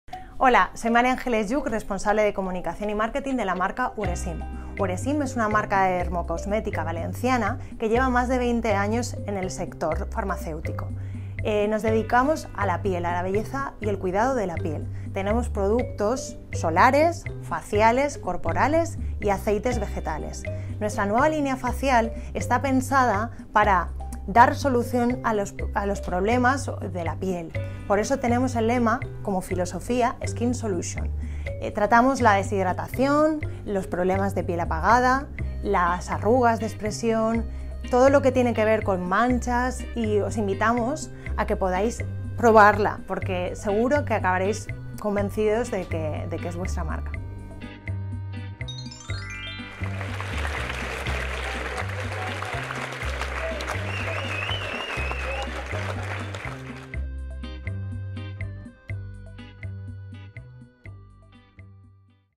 Reto 60 segundos - Uresim | DosFarma
Mª Ángeles Lluch, Responsable de Comunicación y Marketing de Uresim, se enfrenta a explicarlos la marca en tan solo 60 segundos. ¿Lo conseguirá?